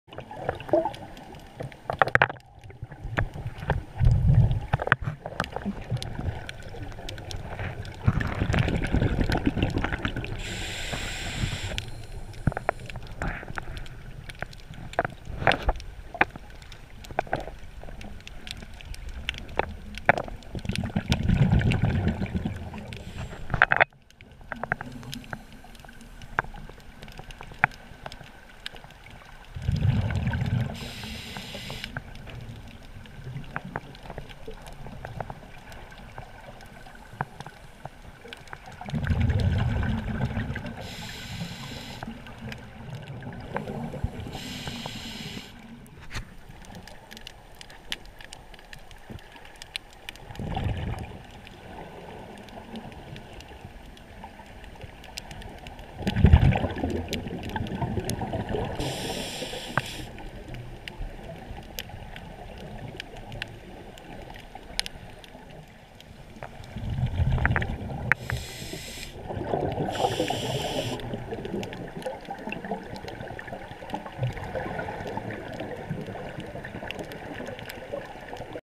Rye Pier (Victoria,Australia)
Filmed on the 31st August 2014, Rye Pier (Victoria)
Quick montage from our dive at the weekend